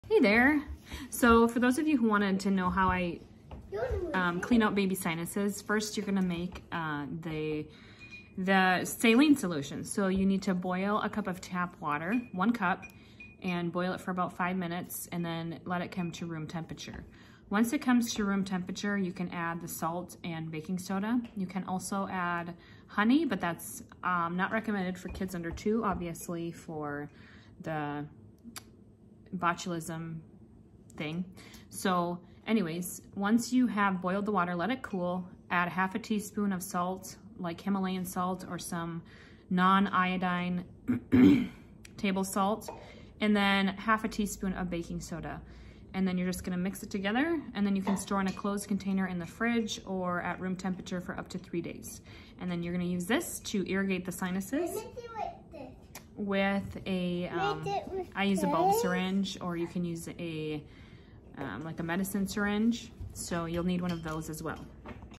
How to make Homemade Saline
How to make homemade saline for flushing baby’s sinuses.